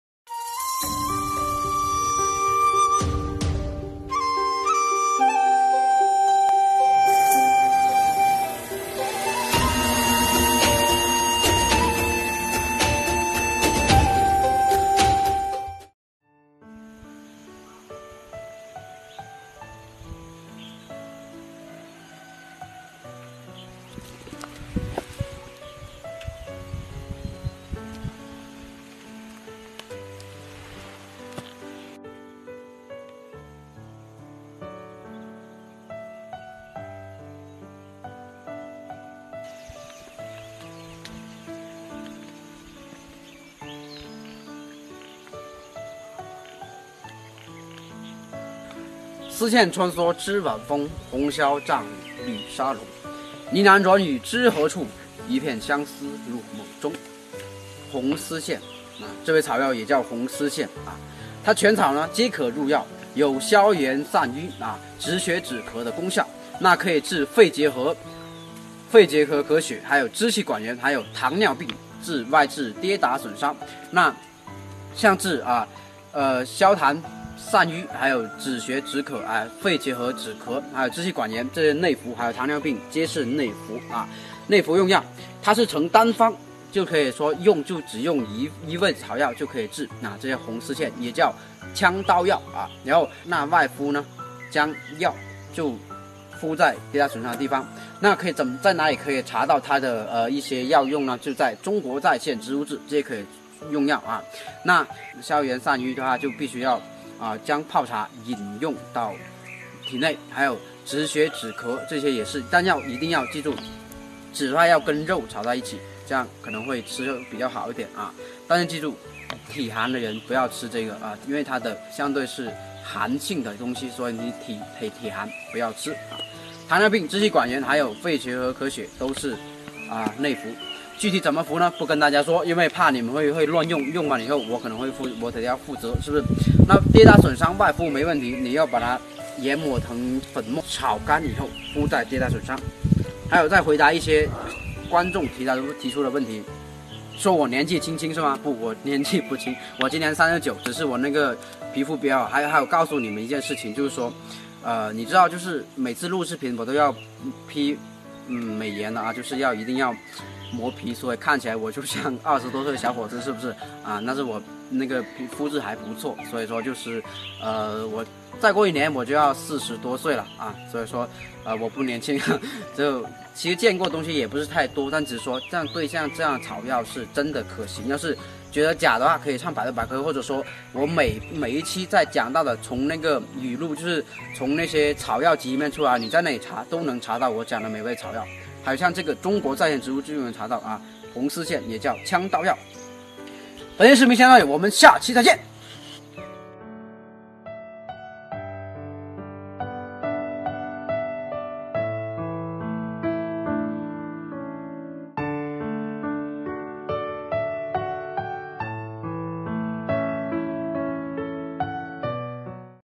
见过农村野草“枪刀药”吗？药用价值极高 - 集草行
红线丝

凡为医道，必先正己，然后正物 正己者，谓明理而尽数也。
青草方中药，苍苔石里钱。